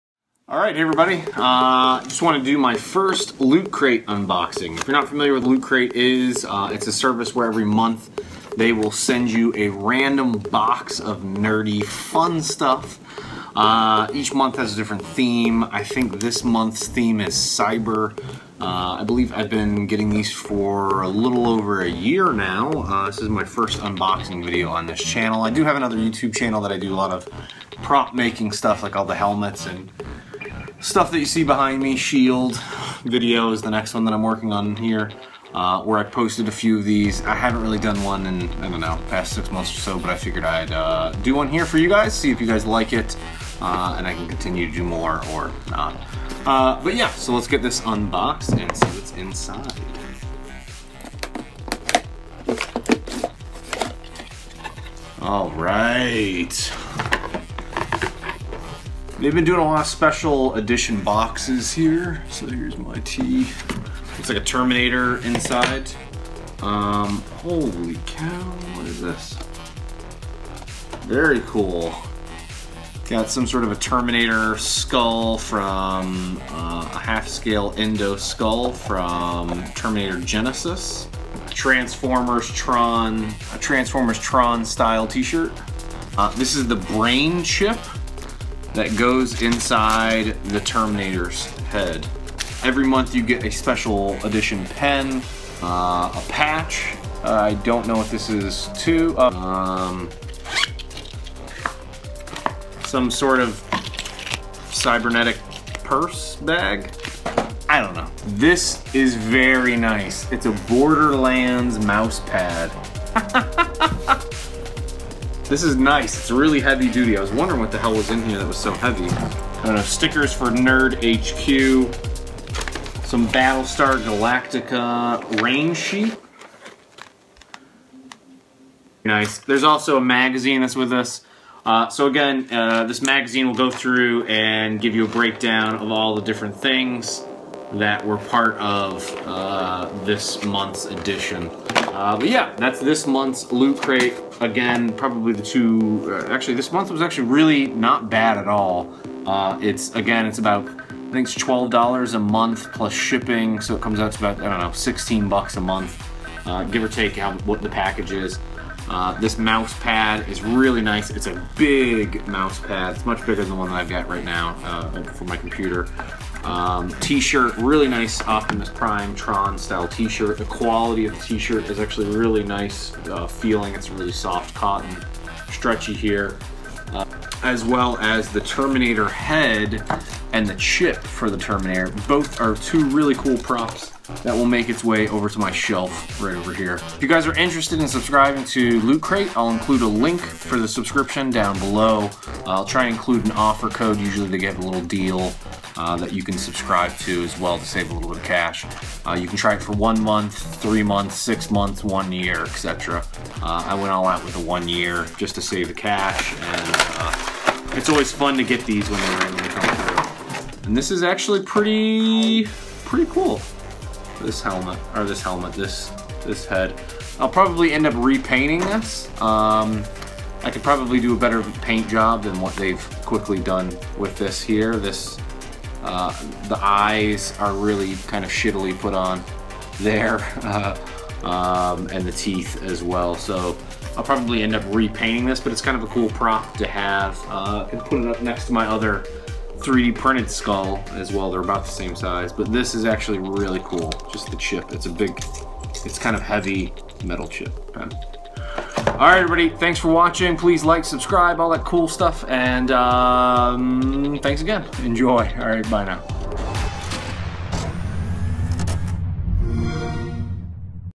Kickass Lootcrate Unboxing June 2015 - Cyber Crate
This is definitely one of the best lootcrates I've gotten so far

I believe we both get a bonus for you signing up

Get your first crate for $10.37 with code APRILLOOT
Save 10% on your order with code SCAR

Next months theme is Heroes 2 - last years box was kickass. cant wait to see what this one is.